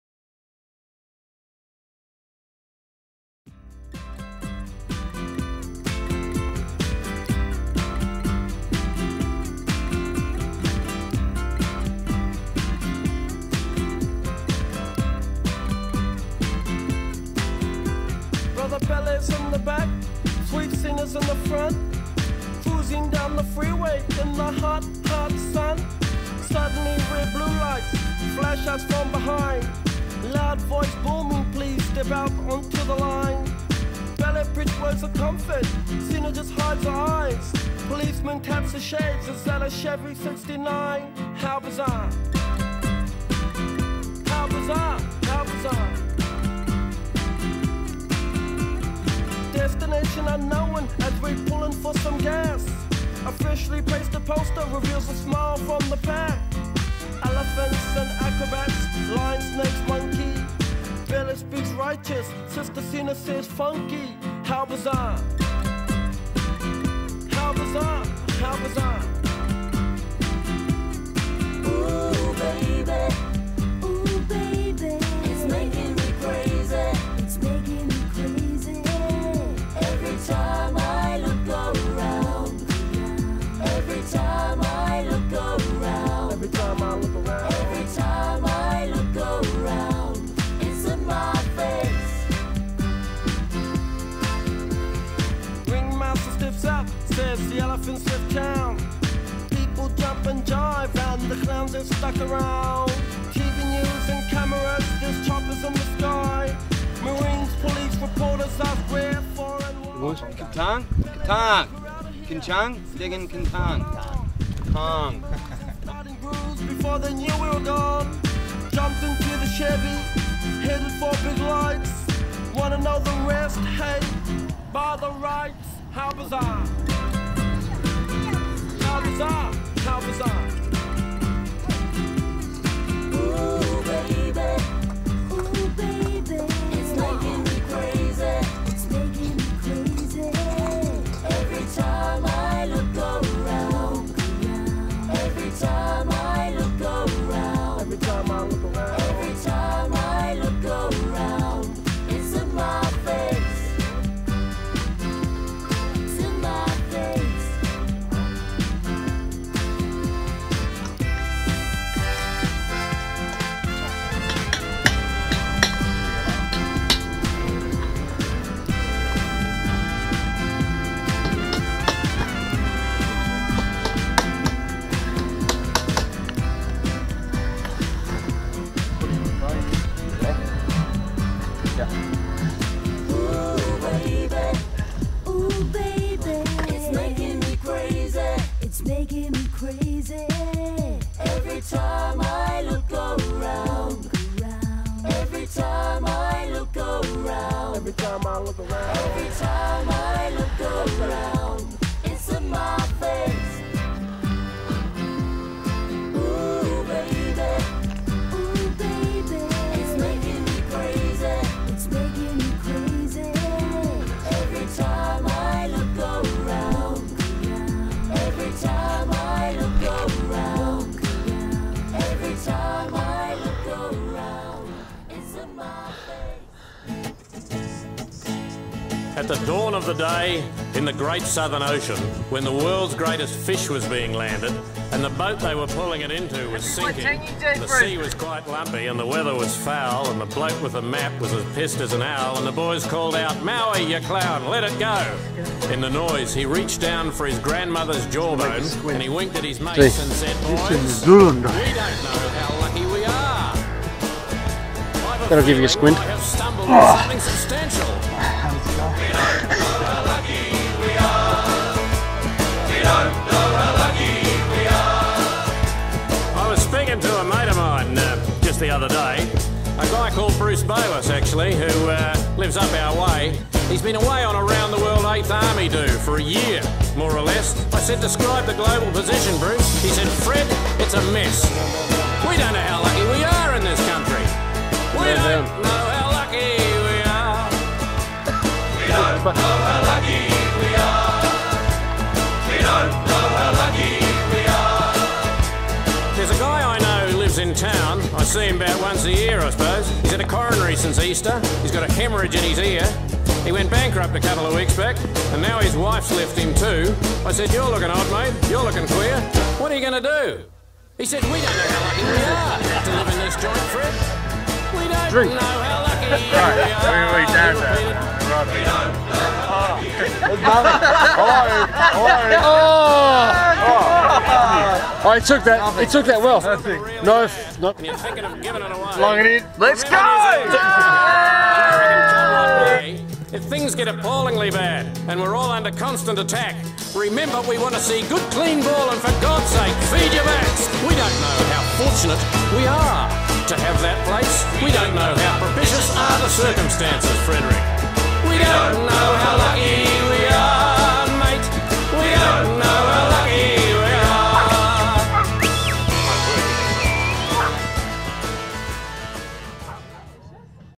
BH4 Waitangi Day Hangi & Hash Run
Batu Hijau Hash House Harriers, Sumbawa, Indonesia
Waitangi Day Hash Run.